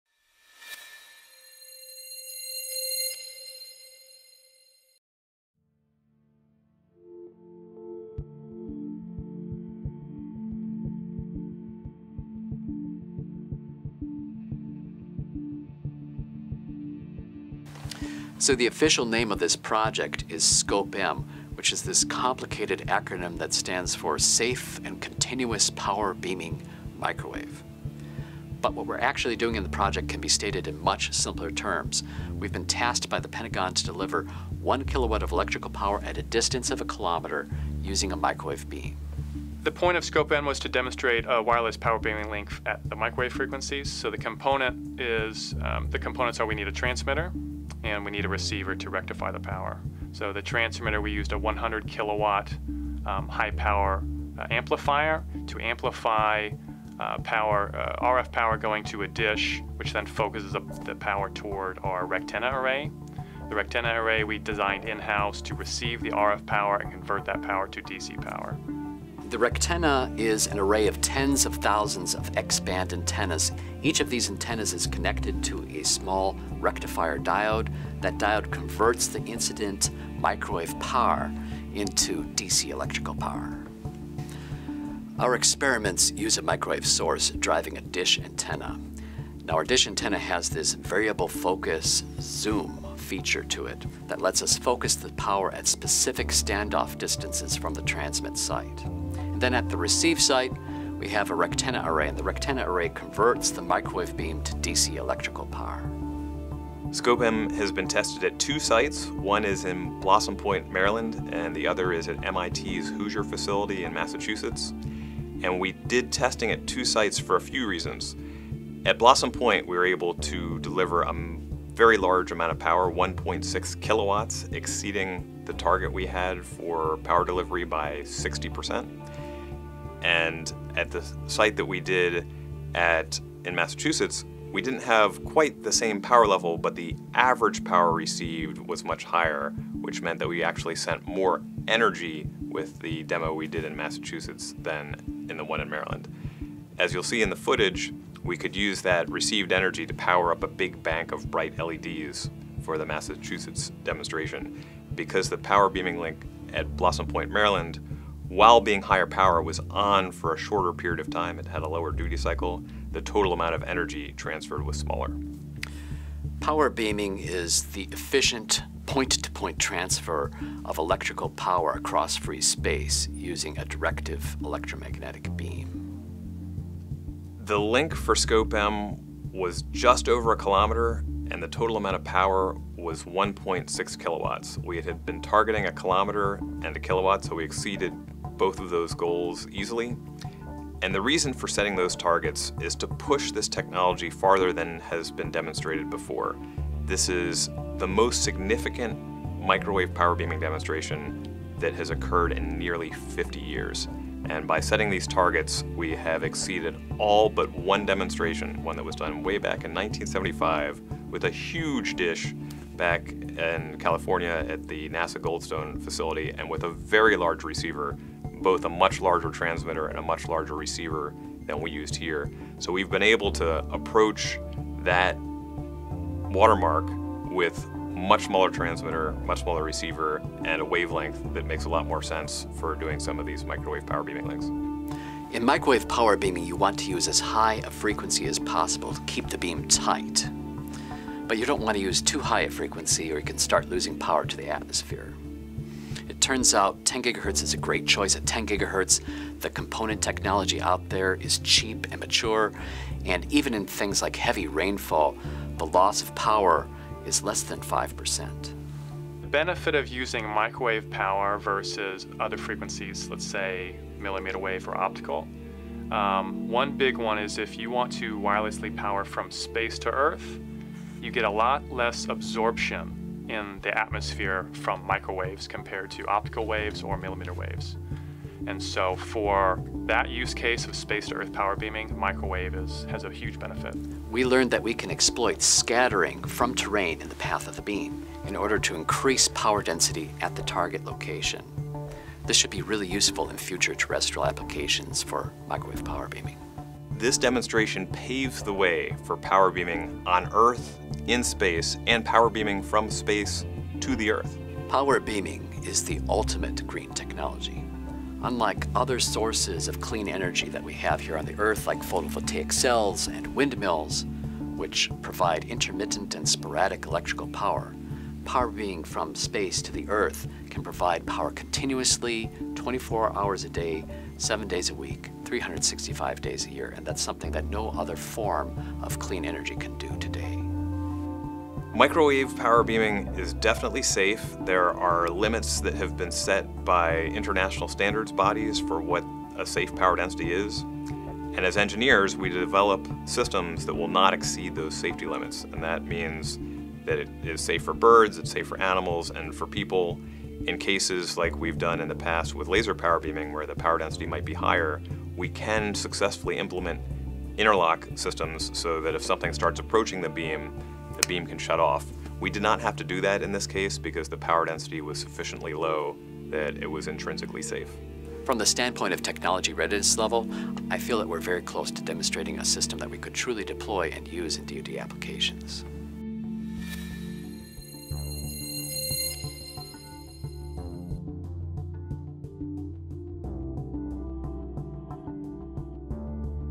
Future of Microwave Power Beaming
Safe and Continuous Power Beaming Microwave (SCOPE-M) is an NRL research project which delivers one kilowatt of electrical power at a distance of a kilometer using a microwave beam. To learn more about this project, go

SCOPE-M is built of tens of thousands of x-band antennas. Each of these antennas is connected to a small rectifier diode that diode converts the incident microwave power into DC electrical power. This demonstration paves the way for power beaming on the earth and from space to the earth.